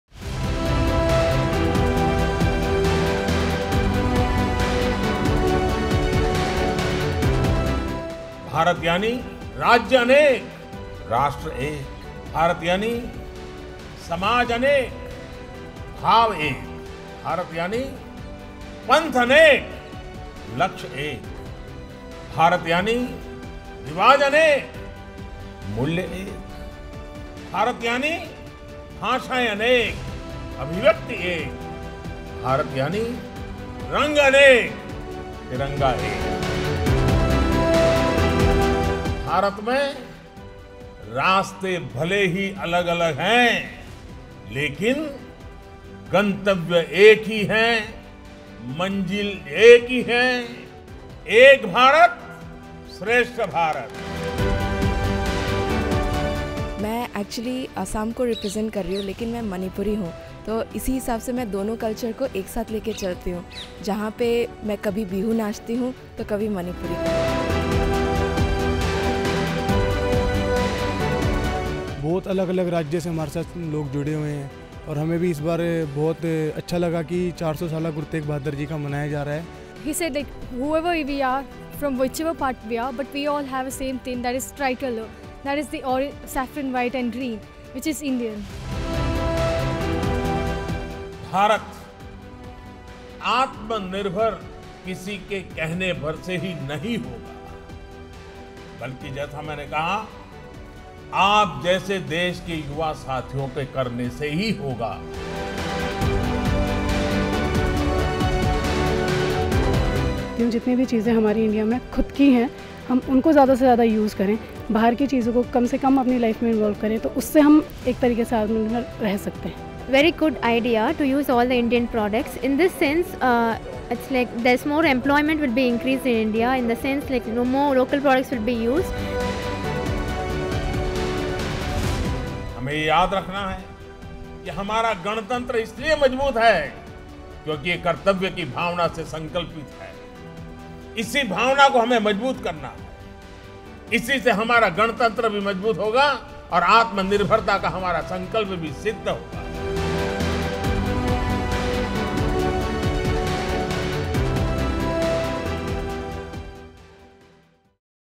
PM Modi meets NCC cadets, tableaux artists, tribal guests taking part in Republic Day parade
PM Modi interacted in an ‘At Home’ event with Tribal Guests, NCC Cadets, NSS Volunteers and Tableaux Artists who would be a part of the upcoming Republic Day parade. He said India means – many states-one nation, many communities -one emotion,  many paths-one goal, many customs-one value, many languages-one expression and many colours-one tri colour. And this common destination is ‘Ek Bharat-Shreshtha Bharat’.

Prime Minister Narendra Modi hosts special guests at his residence ahead of Republic Day parade in Delhi